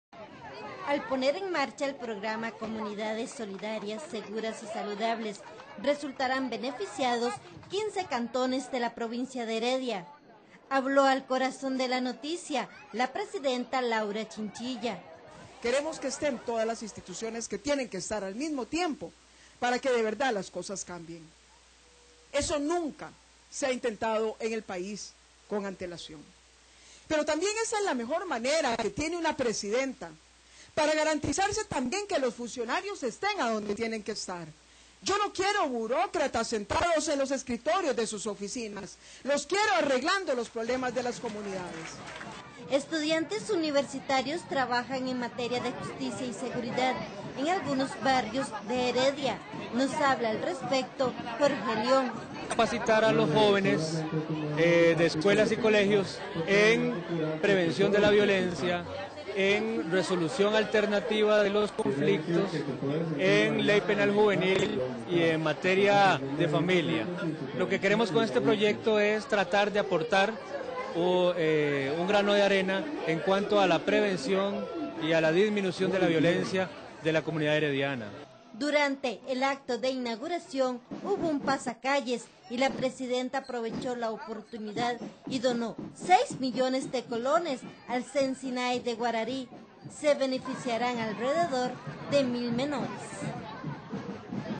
laura heredia
La Presidenta de la República, Laura Chinchilla, visitó la zona de Guarari en la provincia de Heredia, donde inauguró el Programa Comunidades Solidarias, Seguras y Saludables.